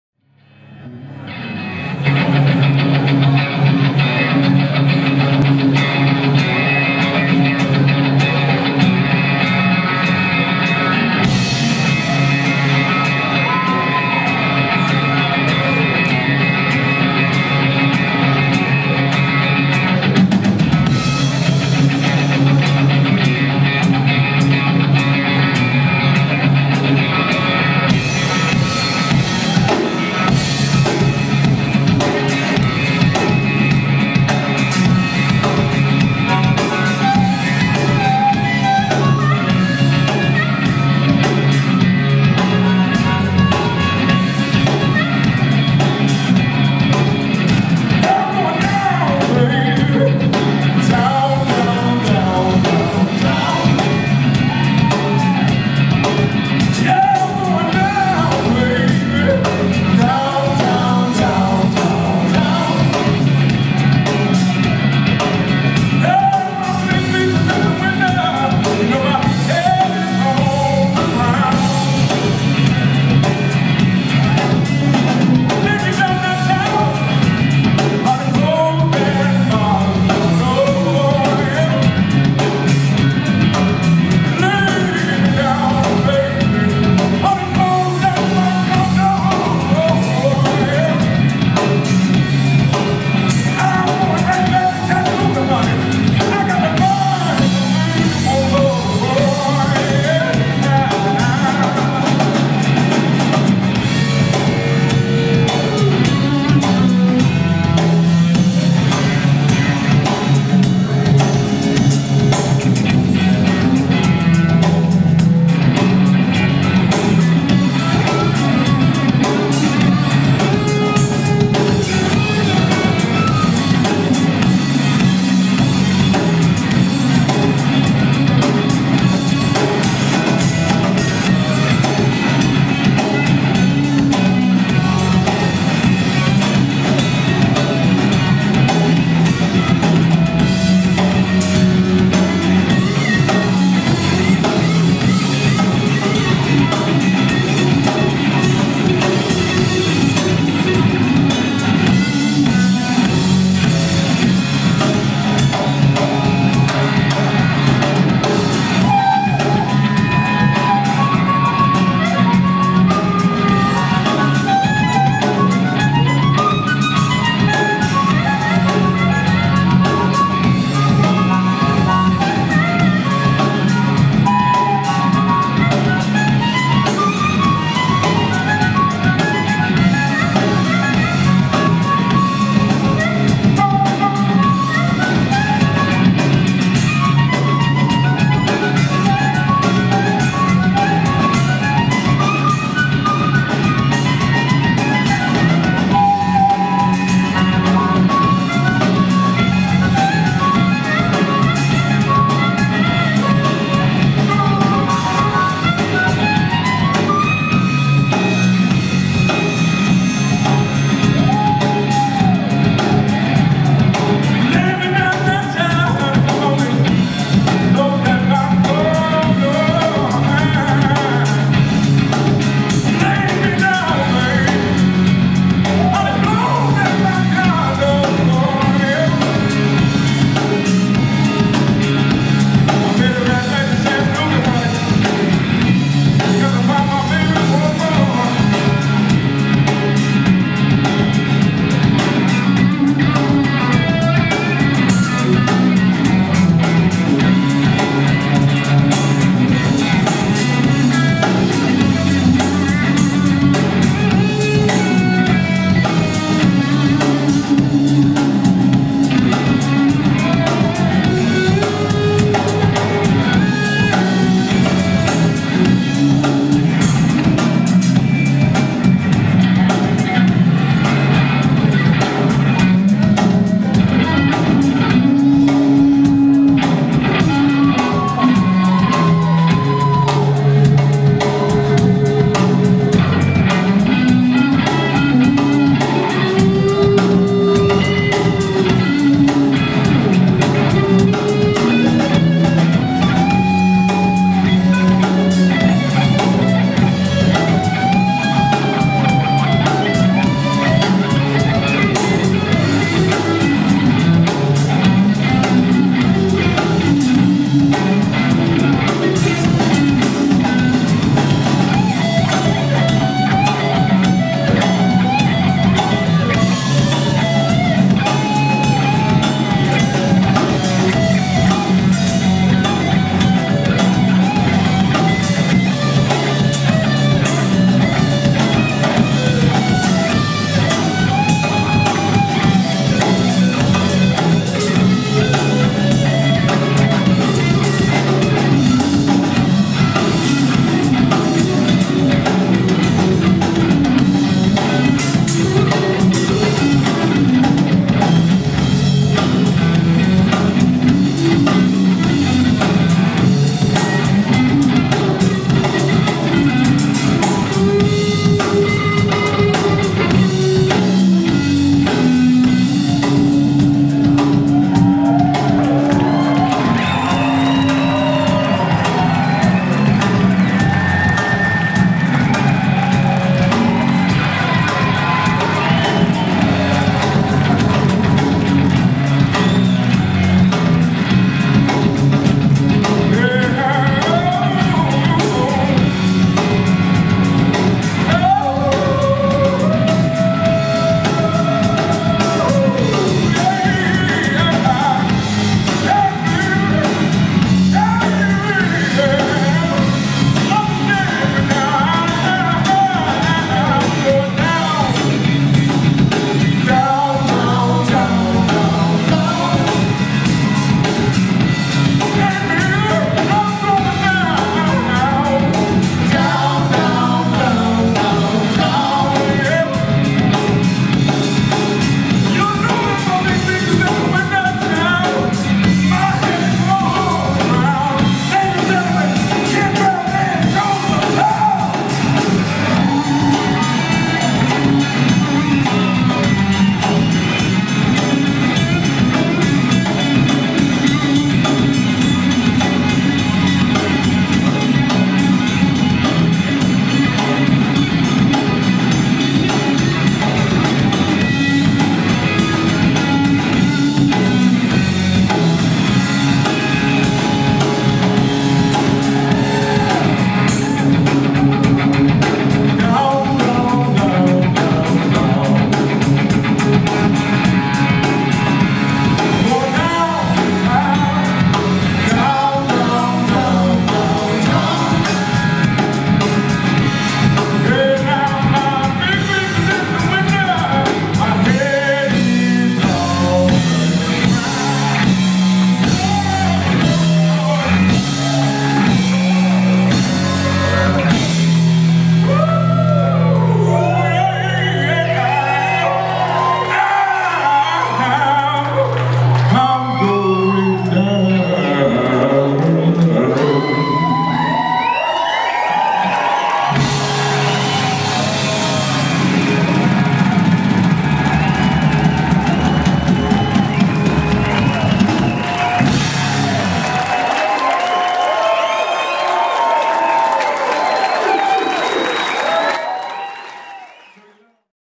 Ken Valdez featuring Kevin "B.F." Burt-- 2018 CIBS Winter Blues Fest
Kevin "B.F." Burt stopped by to join the Ken Valdez Band at the Central Iowa Blues Society Winter Blues Fest at the downtown Marriott, Des Moines.  February 10, 2018